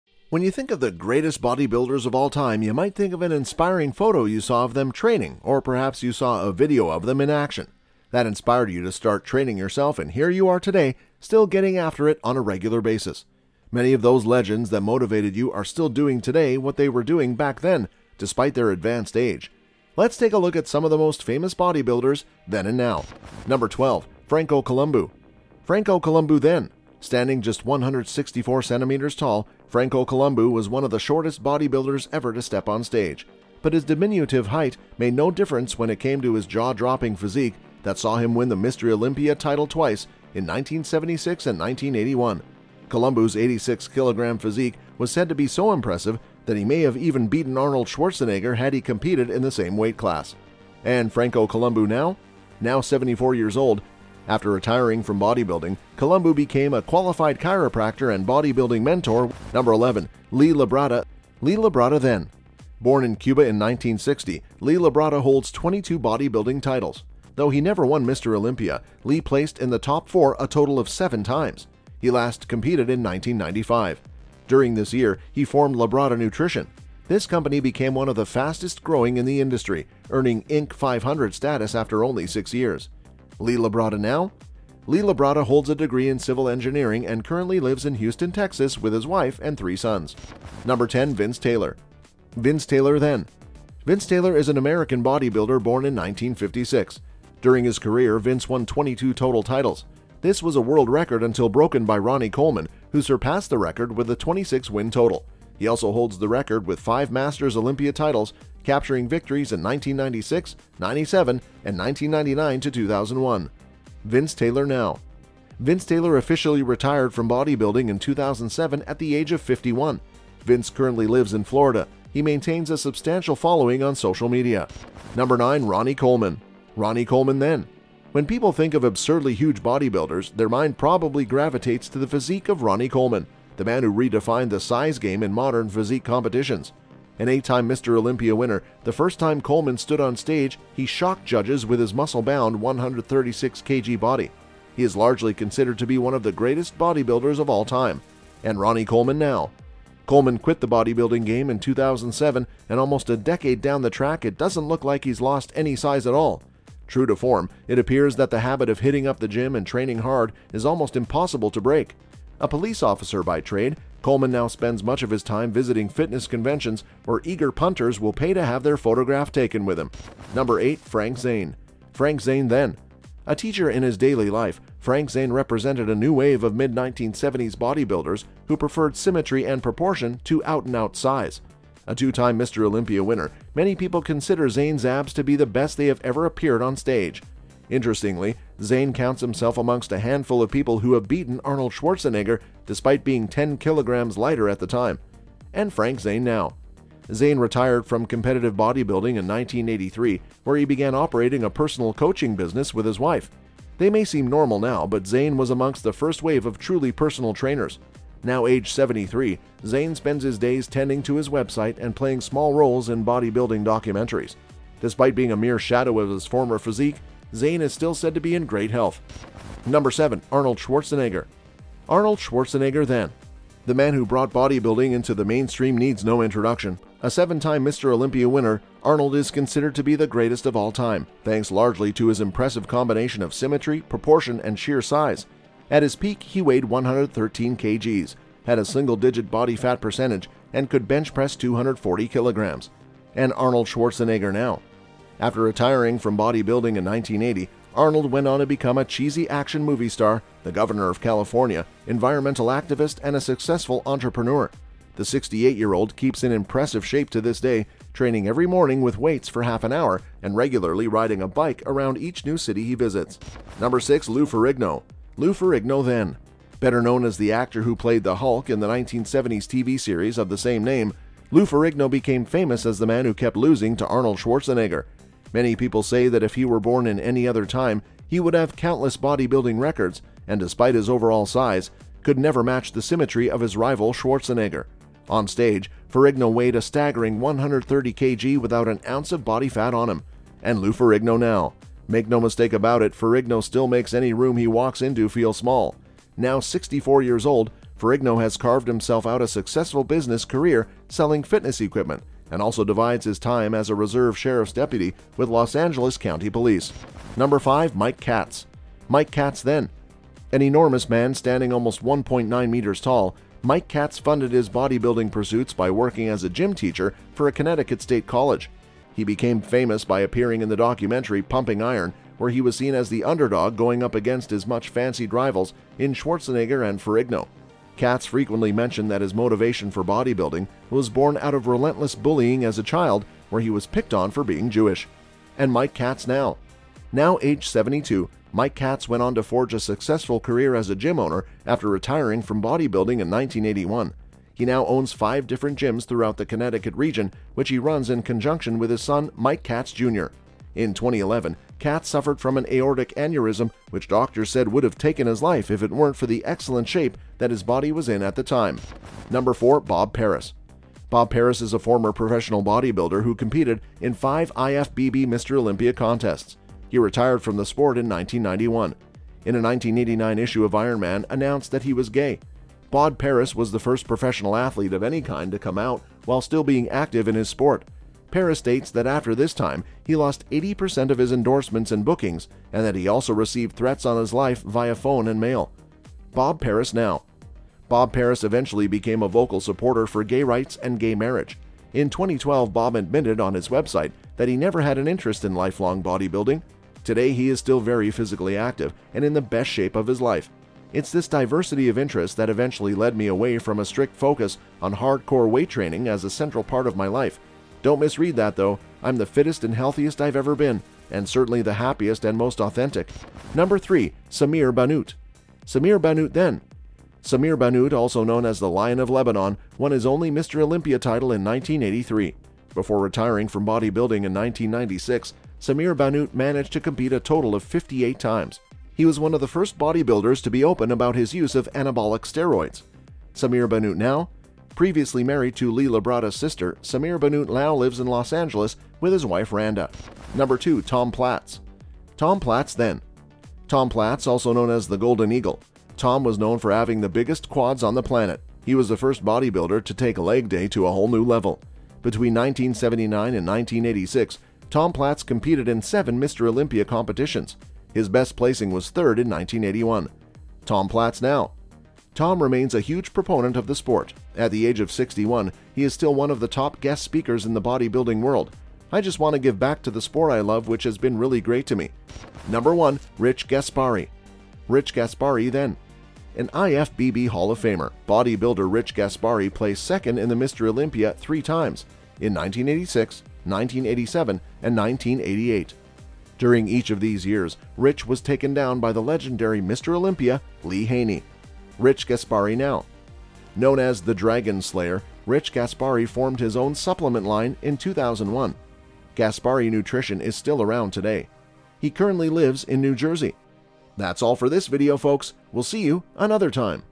TOP 12 Famous Bodybuilders Then and Now
Number 12. Franco Columbu

Franco Columbu then...
Standing just 164 centimetres tall, Franco Columbu was one of the shortest bodybuilders ever to step on stage. But his diminutive height made no difference when it came to his jaw-dropping physique that saw him win the Mr Olympia title twice, in 1976 and 1981. Columbu's 86 kg physique was said to be so impressive that he may have even beaten Arnold Schwarzenegger, had he competed in the same weightclass.

...and Franco Columbu now.
Now 74 years old, Columbu continues to enjoy good health - no doubt due to his years of resistance training and ultra-clean diet. After retiring from bodybuilding, Columbu became a qualified chiropractor and bodybuilding mentor, which he continues to this day.

Number 11. Lee Labrada Then and Now

Lee Labrada then..
Born in Cuba in 1960, Lee Labrada holds 22 bodybuilding titles. Though he never won Mr. Olympia, Lee placed in the top four a total of seven times. He last competed in 1995. During this year, he formed Labrada Nutrition. This company became one of the fastest growing in the industry, earning Inc. 500 status after only six years.

...Lee Labrada now.
Lee Labrada holds a degree in civil engineering and currently lives in Houston, Texas, with his wife and three sons.

Number 10. Vince Taylor

Vince Taylor then..
Vince Taylor is an American bodybuilder born in 1956. During his career, Vince won 22 total titles. This was a world record until broken by Ronnie Coleman, who surpassed the record with a 26 win total. He also holds the record with five masters Olympia titles, capturing victories in 1996, 1997, and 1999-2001.

...Vince Taylor now.
Vince Taylor officially retired from bodybuilding in 2007 at the age of 51. Vince currently lives in Florida. He maintains a substantial following on social media.

Number 9. Ronnie Coleman

Ronnie Coleman then...
When people think of absurdly huge bodybuilders, their mind probably gravitates to the physique of Ronnie Coleman - the man who redefined the "size" game in modern physique competitions. An eight-time Mr Olympia winner, the first time Coleman stood on stage he shocked judges with his muscle-bound 136 kg body. He is largely considered to be one of the greatest bodybuilders of all time.

...and Ronnie Coleman now.
Coleman quit the bodybuilding game in 2007, and almost a decade down the track it doesn't look like he's lost any size at all. True to form, it appears that the habit of hitting up the gym and training hard is almost impossible to break. A police-officer by trade, Coleman now spends much of his time visiting fitness conventions where eager punters will pay to have their photograph taken with him.

Number 8. Frank Zane

Frank Zane then...
A teacher in his daily life, Frank Zane represented a new wave of mid-1970s bodybuilders who preferred symmetry and proportion to out-and-out size. A two-time Mr Olympia winner, many people consider Zane's abs to be the best that have ever appeared on stage. Interestingly, Zane counts himself amongst a handful of people who have beaten Arnold Schwarzenegger, despite being 10 kilograms lighter at the time.

...and Frank Zane now.
Zane retired from competitive bodybuilding in 1983, where he began operating a personal coaching business with his wife. They may seem normal now, but Zane was amongst the first wave of truly personal trainers. Now aged 73, Zane spends his days tending to his website and playing small roles in bodybuilding documentaries. Despite being a mere shadow of his former physique, Zane is said to still be in great health.

Number 7. Arnold Schwarzenegger

Arnold Schwarzenegger then...
The man who brought bodybuilding into the mainstream needs no introduction. A seven-time Mr Olympia winner, Schwarzenegger is considered to be the greatest of all time, thanks largely to his impressive combination of symmetry, proportion and sheer size. At his peak he weighed 113 kgs, had a single-digit body fat percentage, and could bench press 240 kilograms.

...and Arnold Schwarzenegger now.
After retiring from bodybuilding in 1980, Schwarzenegger went on to become a cheesy action movie star, the Governor of California, environmental activist and a successful entrepreneur. The 68-year-old keeps in impressive shape to this day, training every morning with weights for half an hour, and regularly riding a bike around each new city he visits.